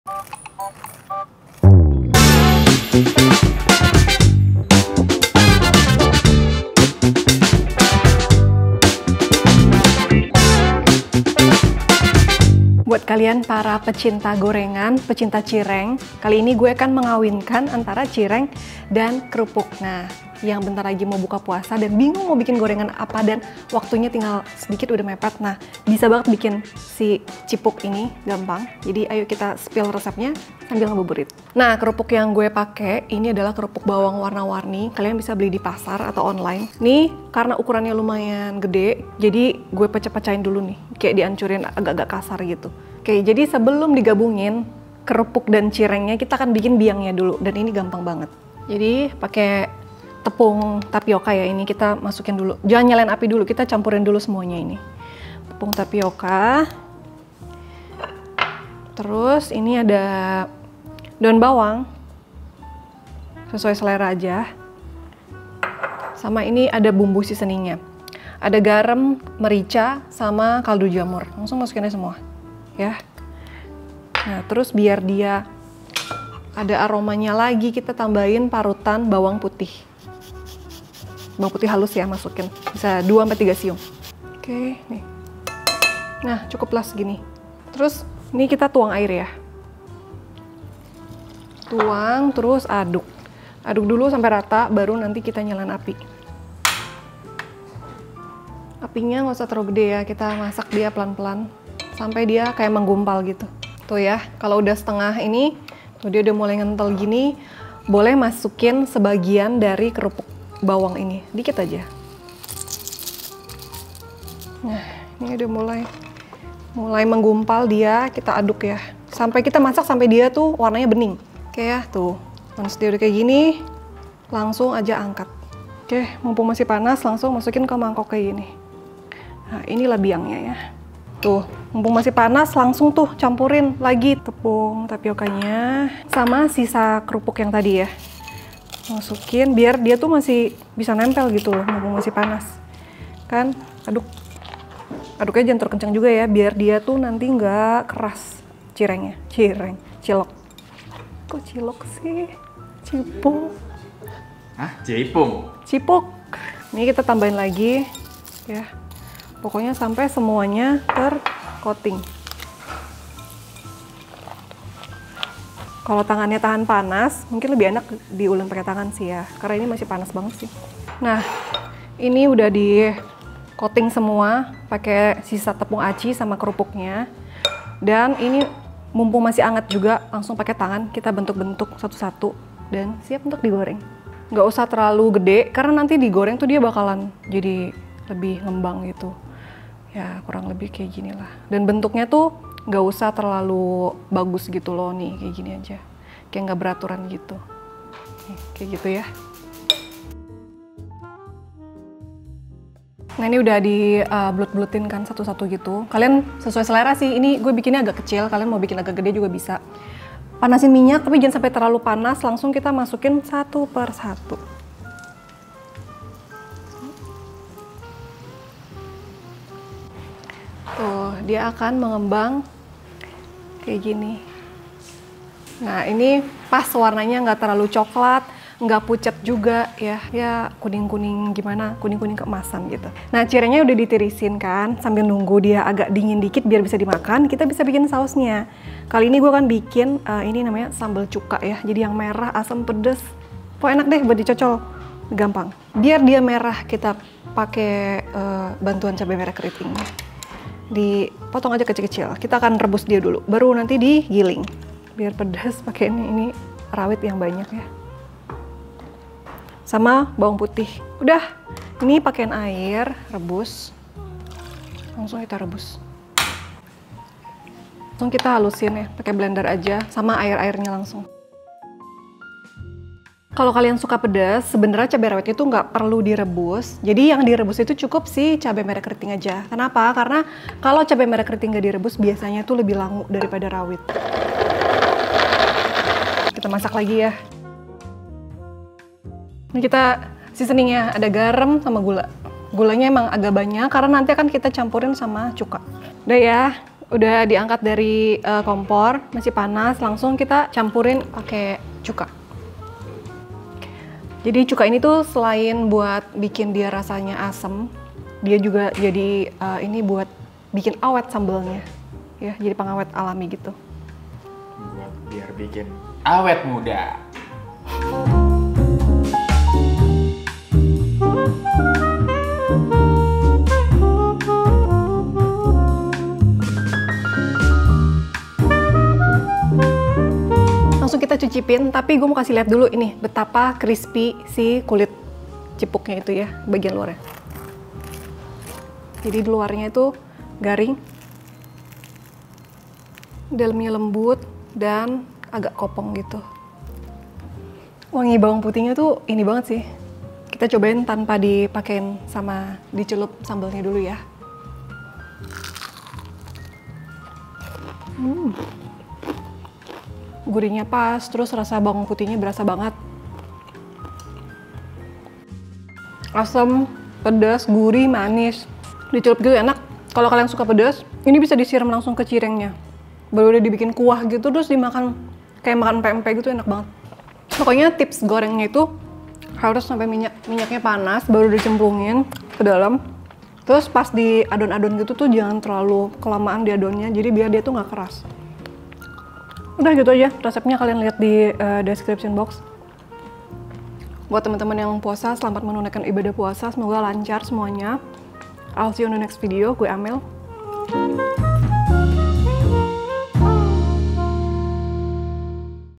RESEP CIRENG KERUPUK UNTUK CEMILAN PAS BERBUKA !
CIPUK alias cireng kerupuknya:

• 5 sdm — tepung tapioka
• 3 siung — bawang putih
• 1 batang — daun bawang
• 1 sdt — garam
• 1 sdt — kaldu bubuk
• 1 sdt — merica
• 250 ml — air

• 200 gr — tepung tapiok
• 150 gr — kerupuk bawang warna-warni

SAMBAL CUKA:

• 10 pcs — cabe rawit merah
• 2 buah — cabe merah keriting
• 2 buah — cabe merah besar
• 2 siung — bawang putih
• 300 ml — air
• 3 sdm — gula pasir
• 1/2 sdt — garam
• 1 sdm — cuka